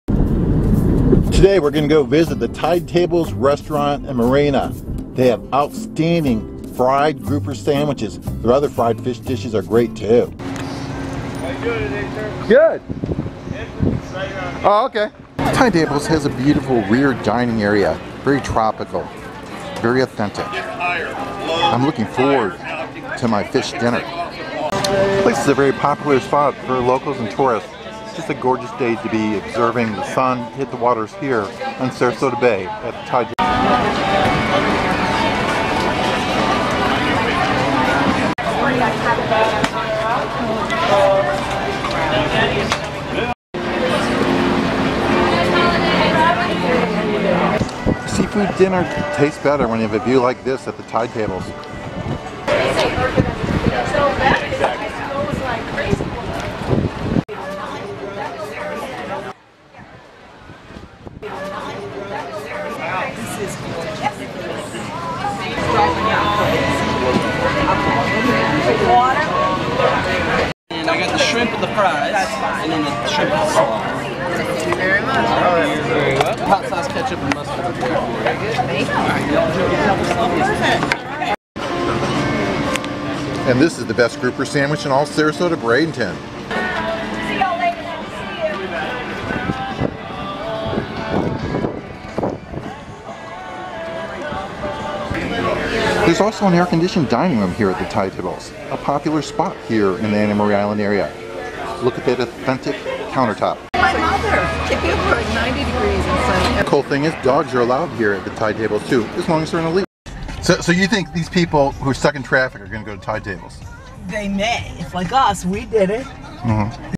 Ultimate Grouper Sandwich Experience: The Perfect Sandwich?
Embark on a mouthwatering journey to Cortez, Florida, as we visit Tide Tables Restaurant & Marina, home to the best grouper sandwiches in town! 🥪🌊

Nestled in the charming fishing village of Cortez, Tide Tables offers delicious seafood, stunning waterfront views, and a laid-back atmosphere. Join us as we explore this hidden gem and indulge in their famous grouper sandwiches made with the freshest locally-sourced ingredients. 🎣🍽️

In this video, we'll give you an insider's look at the restaurant's menu, ambiance, and marina, showcasing the unique charm that makes Tide Tables a must-visit destination for foodies and travelers alike. 🌴🛥️

Timestamps:
(0:00) About Tide Tables Restaurant & Marina
(0:15) Outside Dining
(0:25) Waiting for a Table
(0:35) Outside Bar
(0:45) Bay Views
(0:55) Old Florida
(1:10) What's Good?
(1:22) Grouper Sandwich Taste Test
(1:36) Inside Dining
(1:46) Dogs Are Allowed